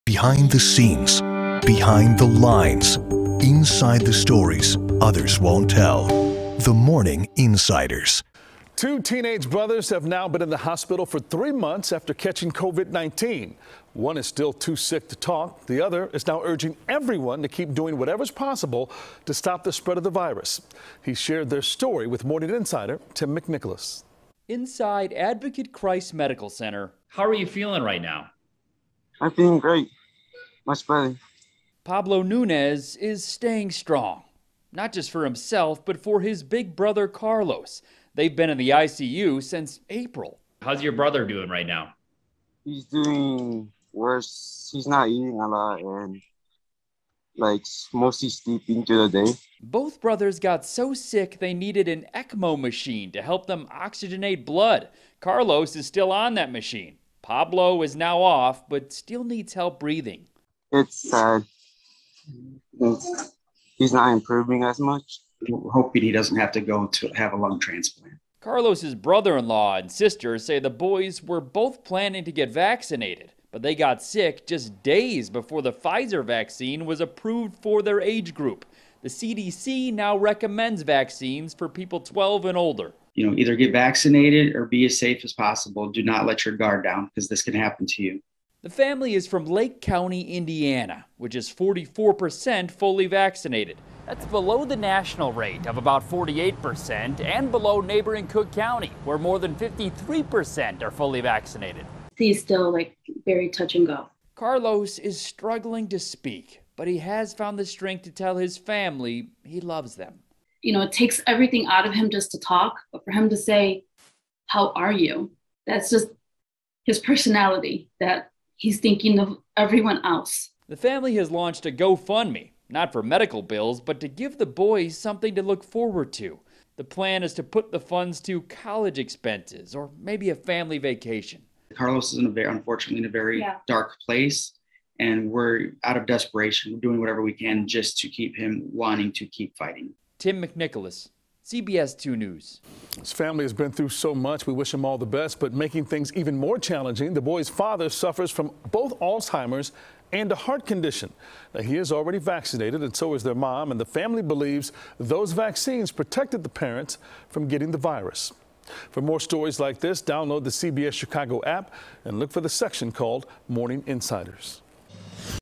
Teen Brothers From Indiana Fighting 3-Month Battle Against COVID-19
One is still too sick to talk, and the other is now urging everyone to keep doing whatever possible to stop the spread of the virus. He shared their story with Morning Insider Tim McNicholas.